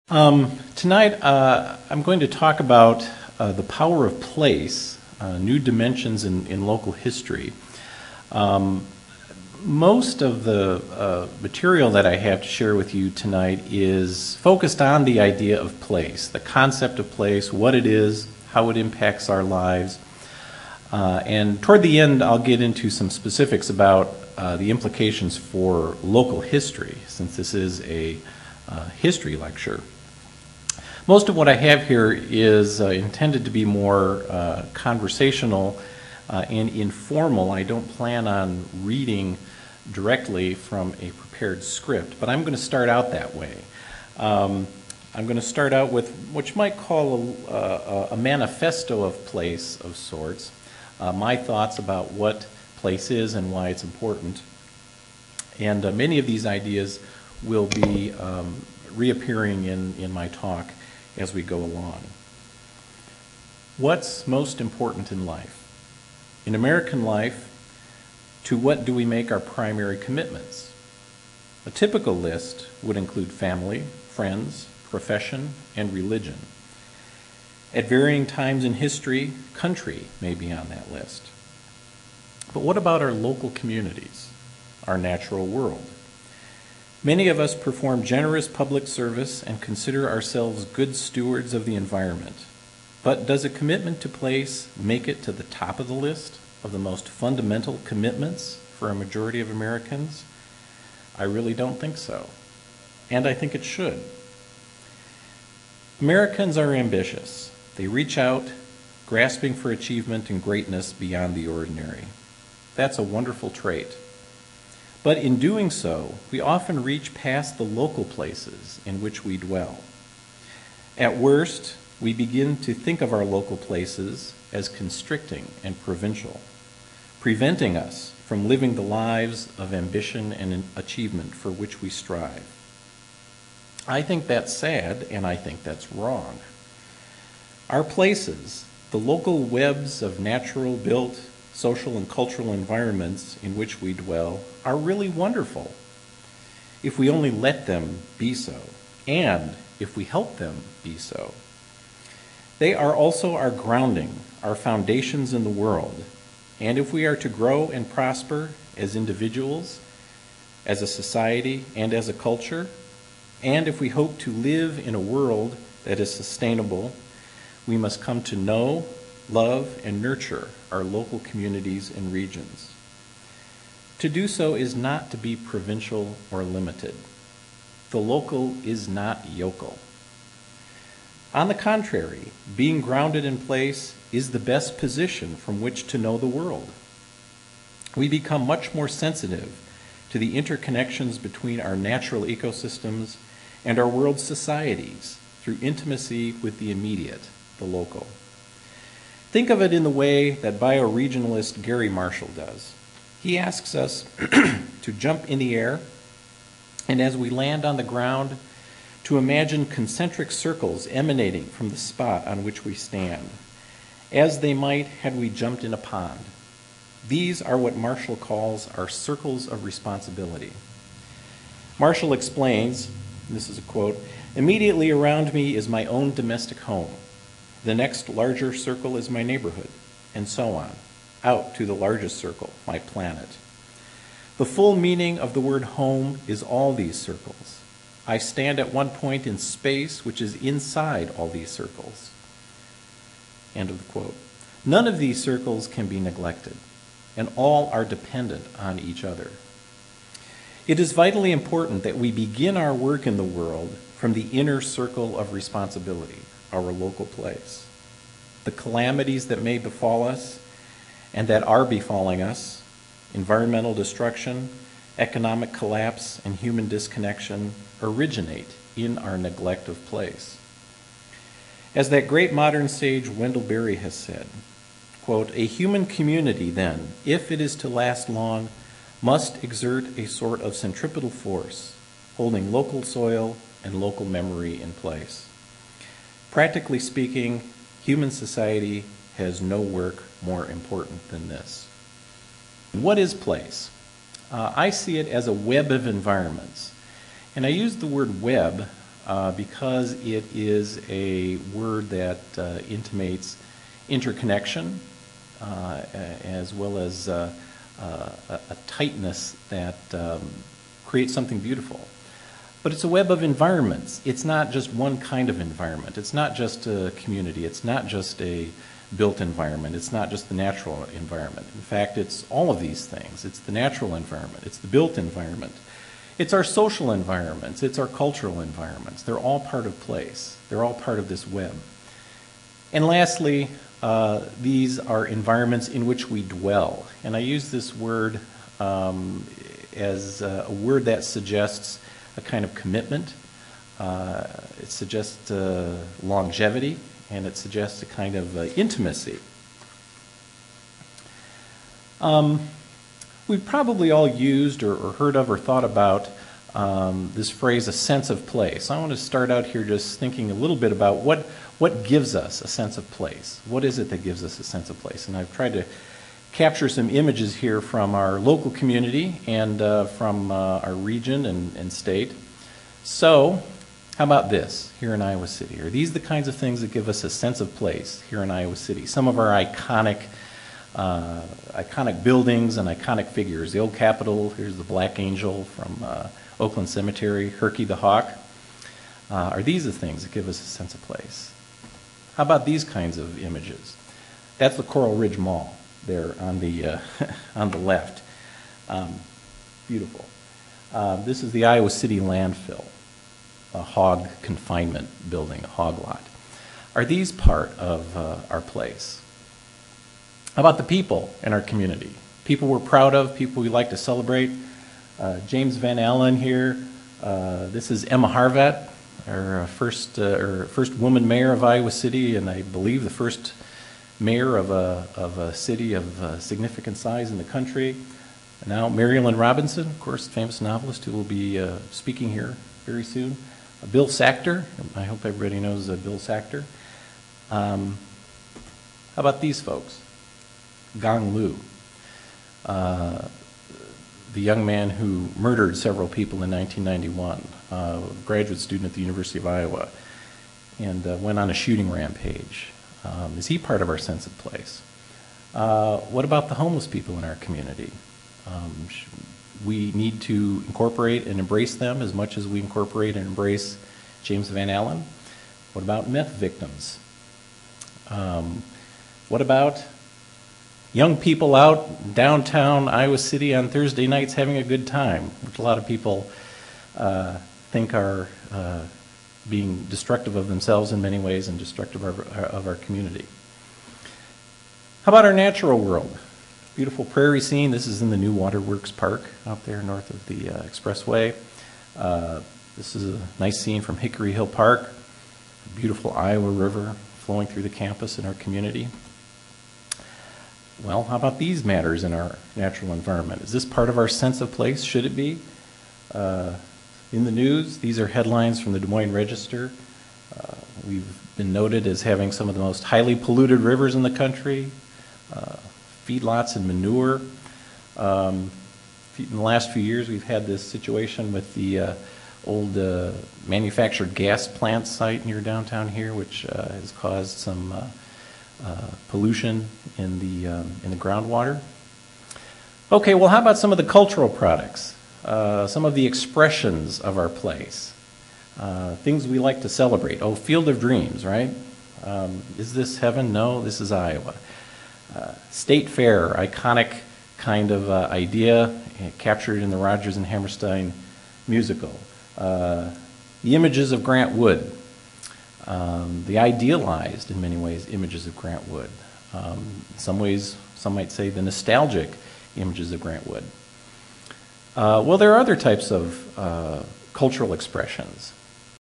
Weber Days History Lecture
Thomas K. Dean presents the 2006 Irving B. Weber Days History Lecture: "The Power of Place: New Dimensions of Local History"

Thomas K. Dean is Special Assistant to the University of Iowa President, one of the Iowa Project on Place Studies coordinators, and an adjunct assistant professor of literature, science and the arts.